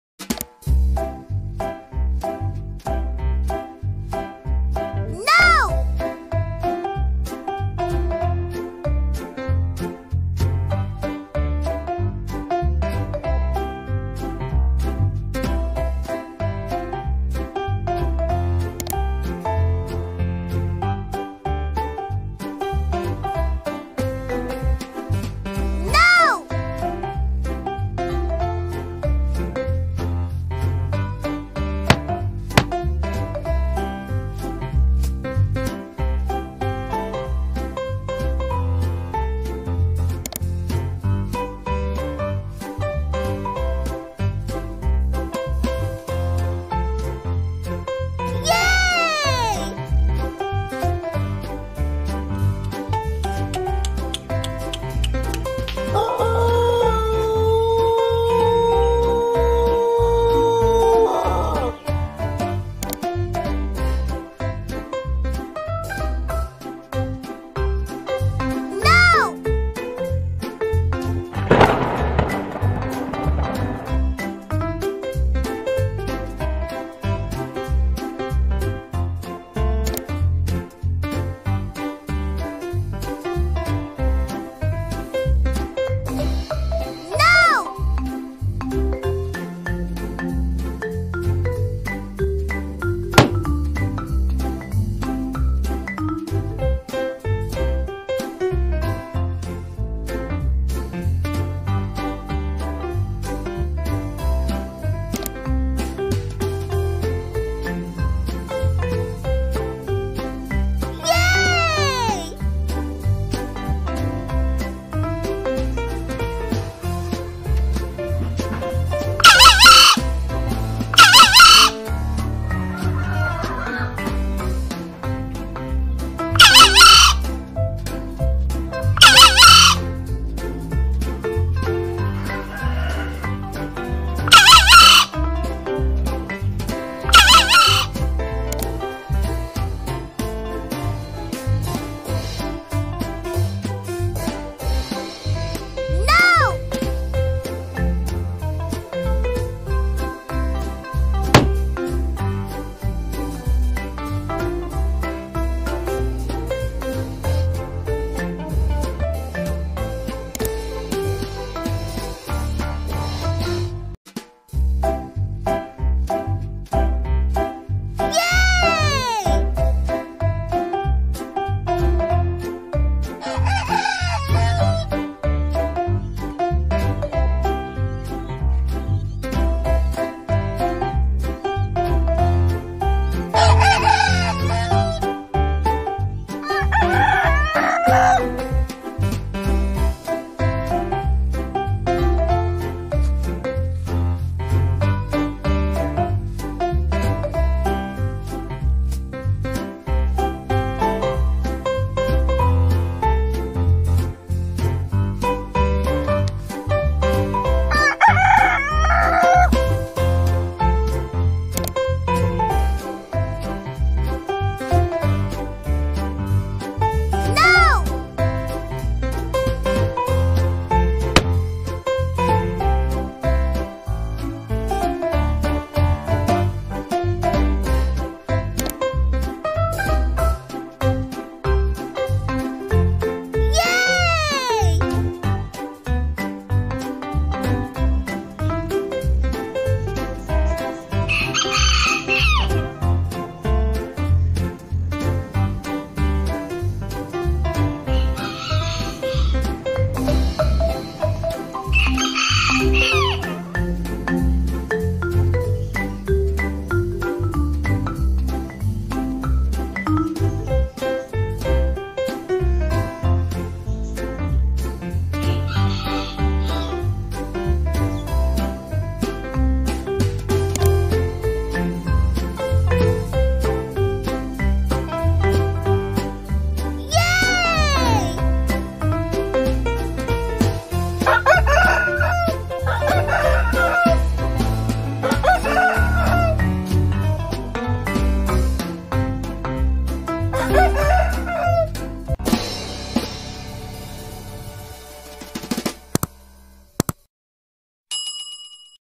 kokok Ayam!! ,Cemani, Pelung,Serama, Ayam Hutan dan menempatkan gambar ayam pada posisinya
Kokok Ayam,Cemani,Pelung,Serama,Ayam hutan, serta menempatak gambar ayam pada posisinya

berbagai macam suara kokok ayam jago mempunyai tipe masing2 dan mempunyai ciri khas sendri